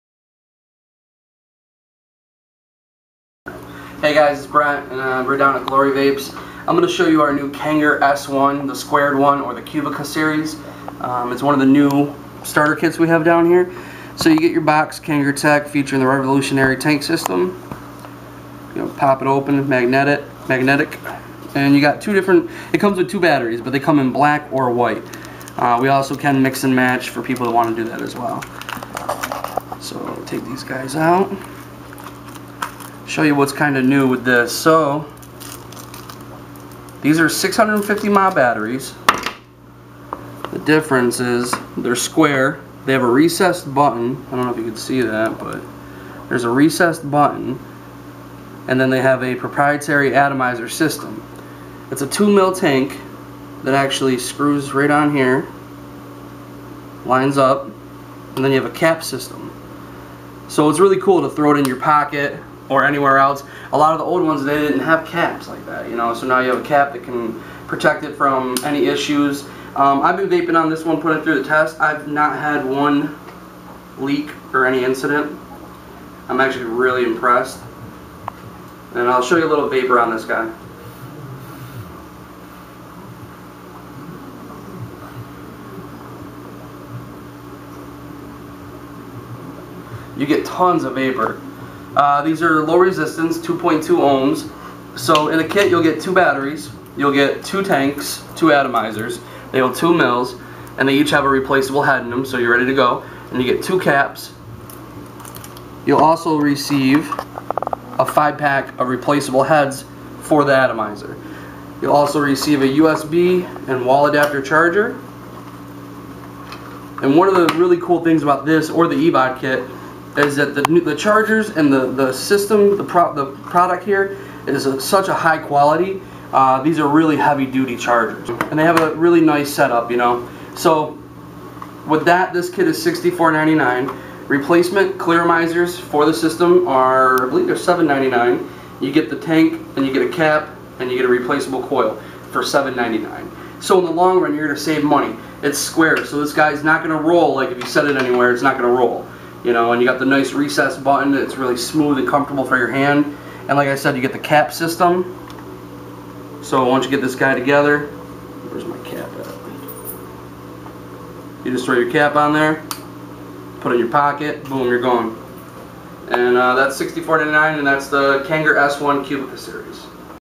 Glory Vapes TV: Kanger S1 Cubica Series Starter Kit Unboxing
Brent shows you the Kanger S1 Cubica Series starter kit, unboxes it, and shows off it's features.